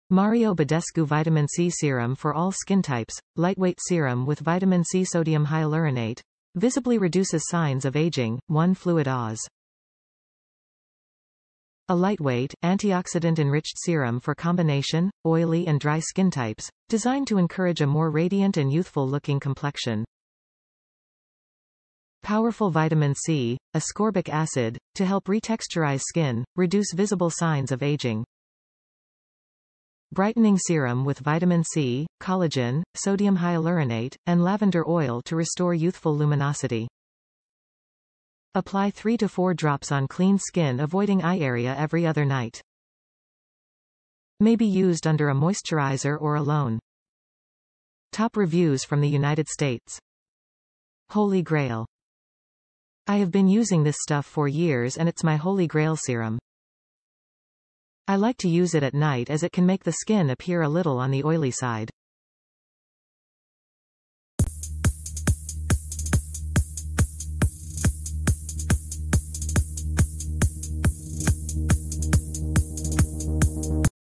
Mario Badescu Vitamin C Serum for All Skin Types | Lightweight Serum with Vitamin C & Sodium
Mario Badescu Vitamin C Serum for All Skin Types | Lightweight Serum with Vitamin C & Sodium Hyaluronate | Visibly Reduces Signs of Aging | 1 Fl Oz...
A lightweight, antioxidant-enriched serum for combination, oily and dry skin types, designed to encourage a more radiant and youthful-looking complexion. Powerful Vitamin C (Ascorbic Acid) to help retexturize skin, reduce visible signs of aging.
Brightening serum with Vitamin C, Collagen, Sodium Hyaluronate, and Lavender Oil to restore youthful luminosity.
Apply 3-4 drops on clean skin avoiding eye area every other night. May be used under a moisturizer or alone.
Holy Grail
I have been using this stuff for years and it’s my holy grail serum. I like to use it at night as it can make the skin appear a little on the oily side.